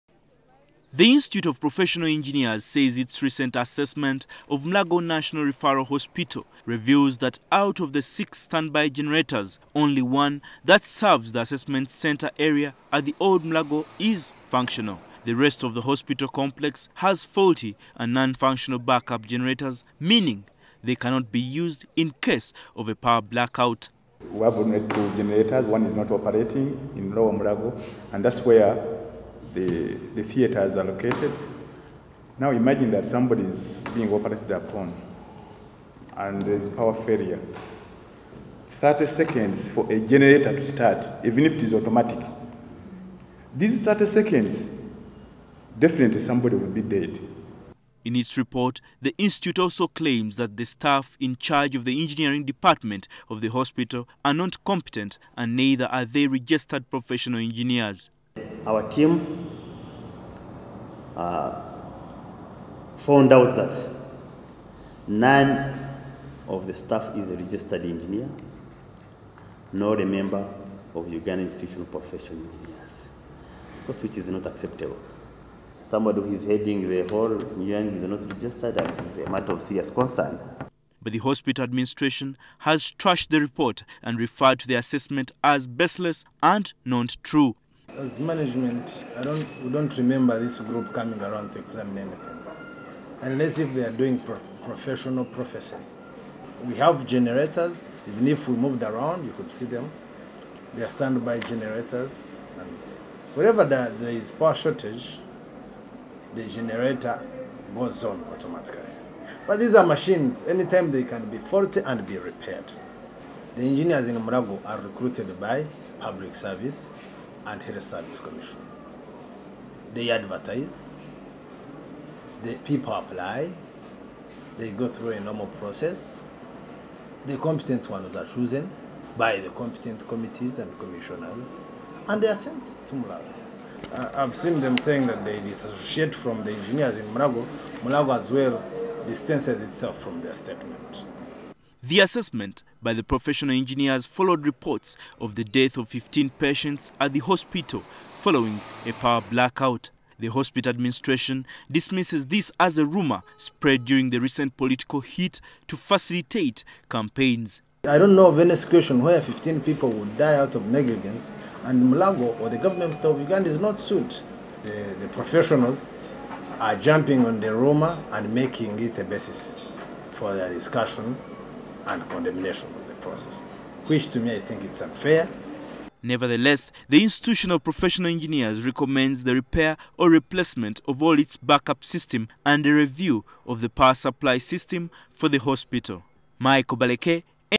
Engineers term Mulago Hosp. a death trap
Mulago hospital could be a death trap; a report released by the institute of professional engineers has revealed. The report follows an assessment of the national referral hospital's back-up power supply, which was found lacking. The engineer's body says in its present state, patients are likely to die while undergoing surgery or intensive care in case of a power outage.